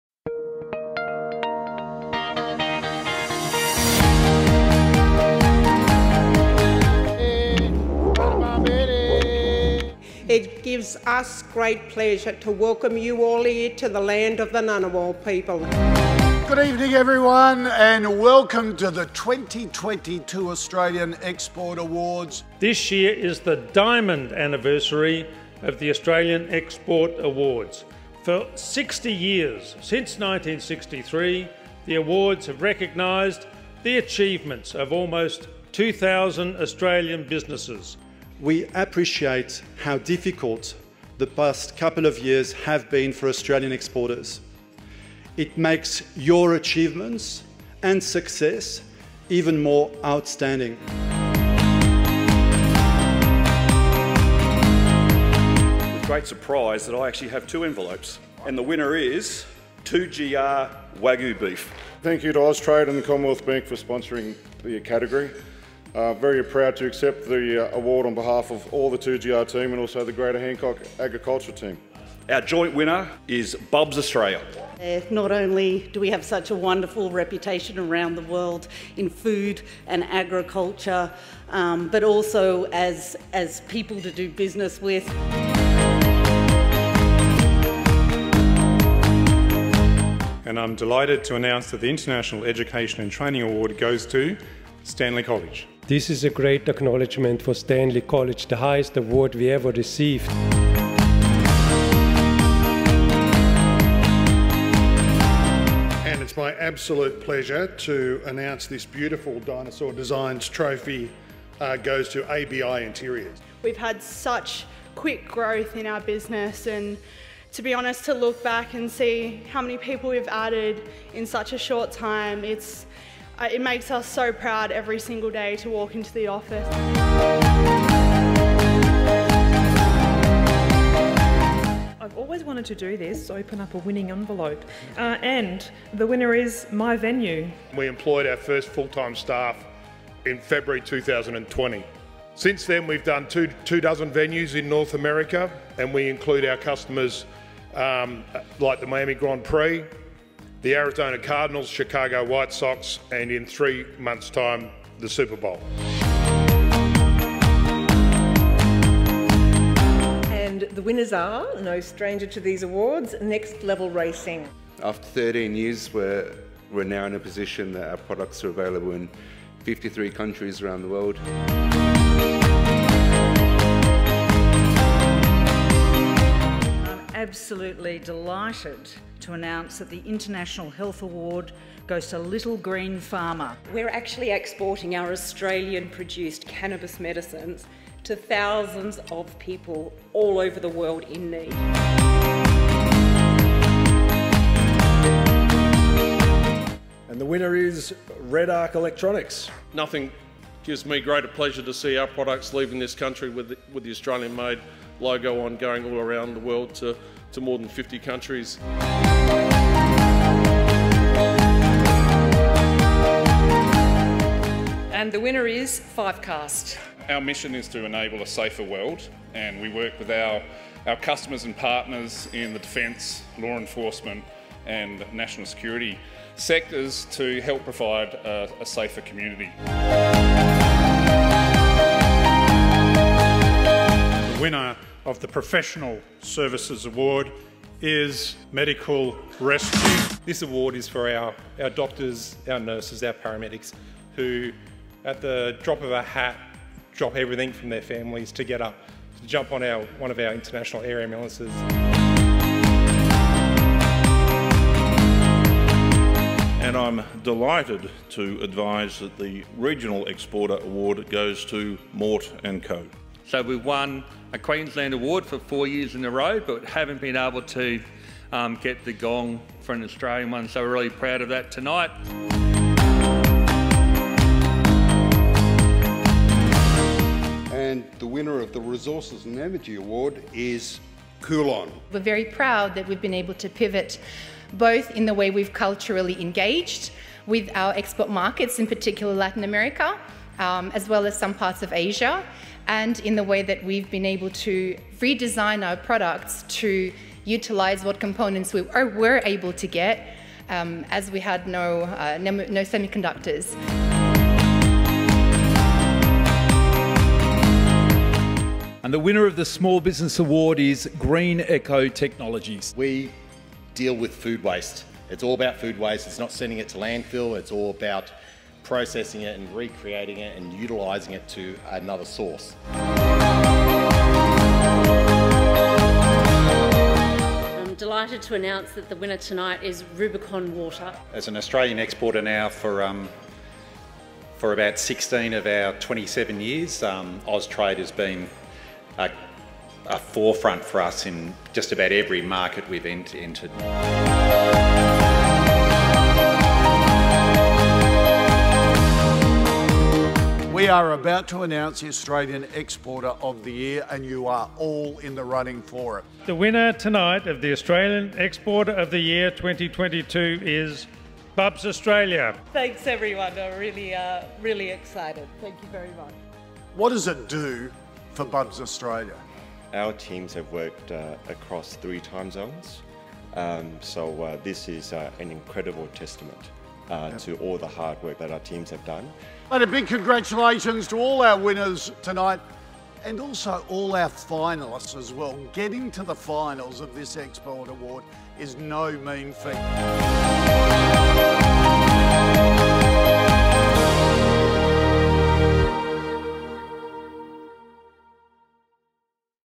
2022 Australian Export Awards - Highlights
The 60th Australian Export Awards ceremony, co-presented by Austrade and the Australian Chamber of Commerce and Industry, was held on Thursday 24 November 2022.

The ceremony was hosted by Senator the Hon Don Farrell, Minister for Trade, Tourism and Investment. The Australian Exporter of the Year for 2022 and 14 national export category winners were announced.